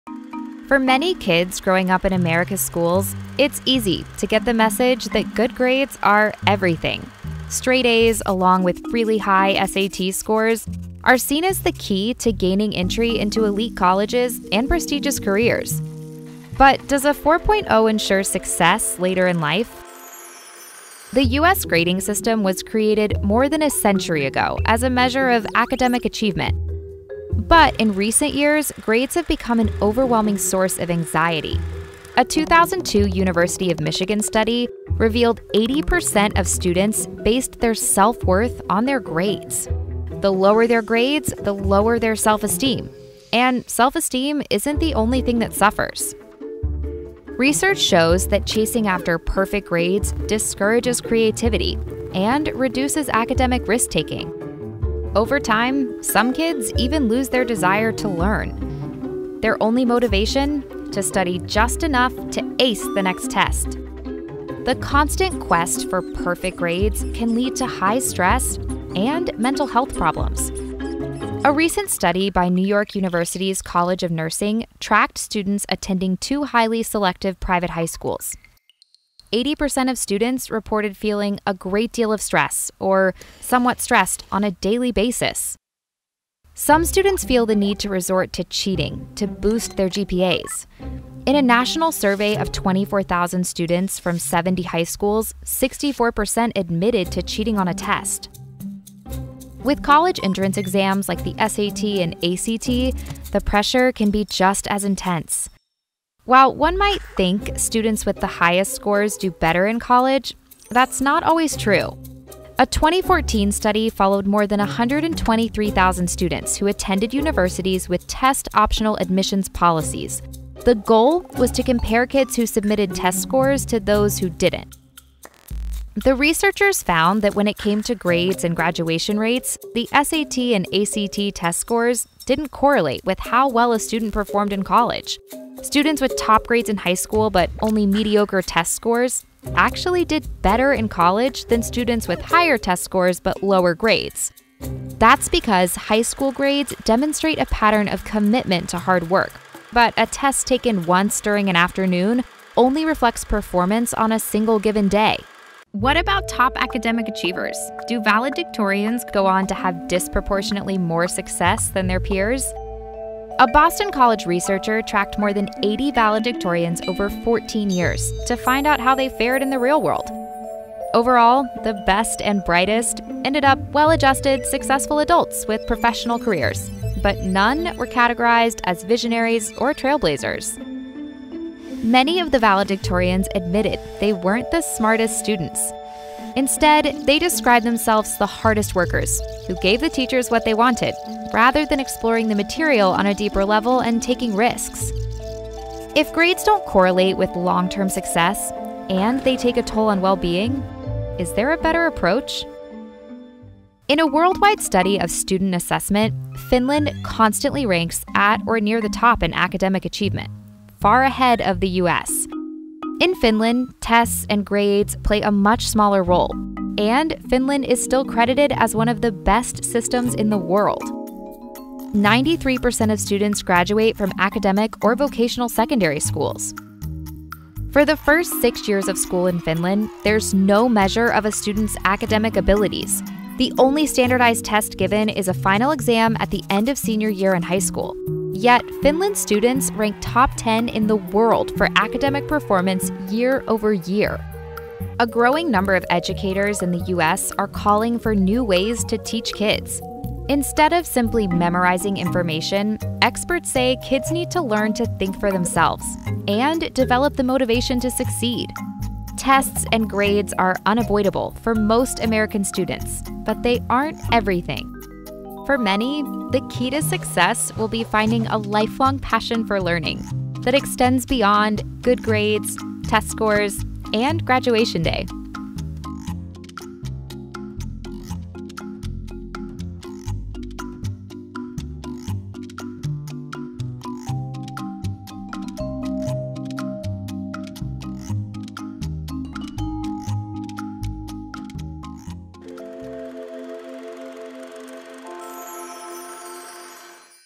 Why Perfect Grades Don't Matter
Research shows that chasing after perfect grades discourages creativity and reduces academic risk-taking. Here's why good grades don't always translate into success in life.